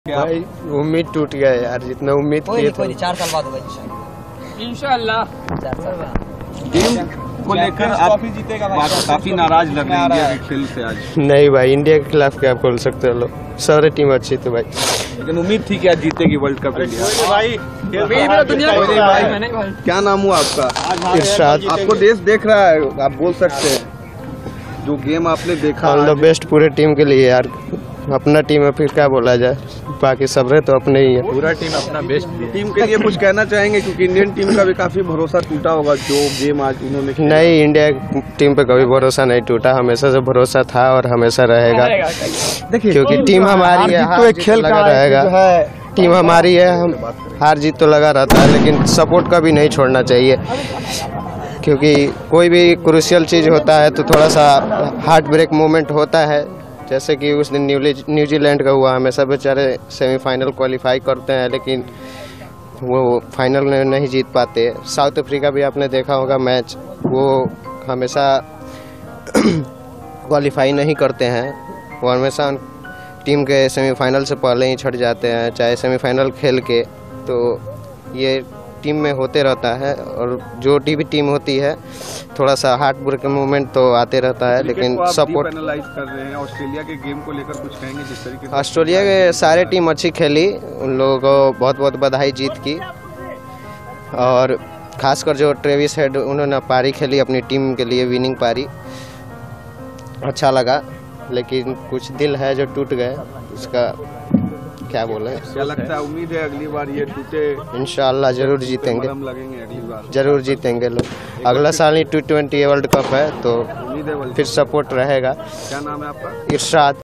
"उम्मीद टूट गया यार" भारत की हार पर लड़के के आंसू देख दिल टूट जाएगा!
"उम्मीद टूट गया यार" | भारत की हार पर लड़के के ये आंसू देख आपका भी दिल टूट जाएगा! | Viral Video | World Cup 2023 | Source ANI